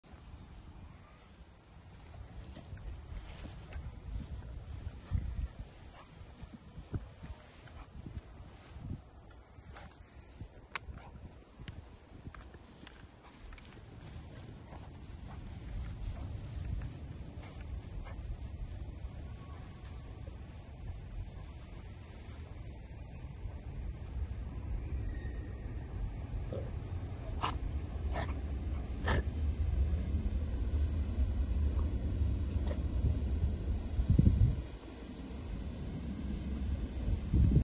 Walter the Clumber Spaniel using his nose
I, Walter, bound around in the long grass snuffling out my toy. I am part rabbit, part Clumber Spaniel!